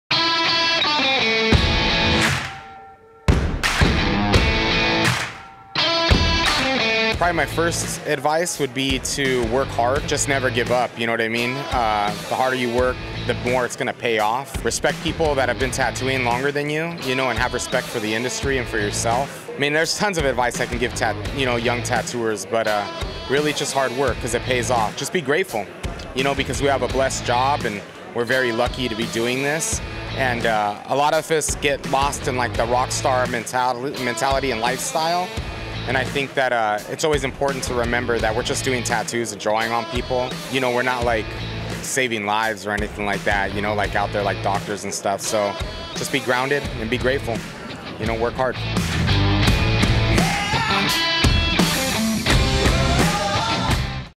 Ethics & Respect - Tattoo Artist Advice by Nikko Hurtado
Tattoo artist Nikko Hurtado - Check out his advice for up and coming tattoo artists!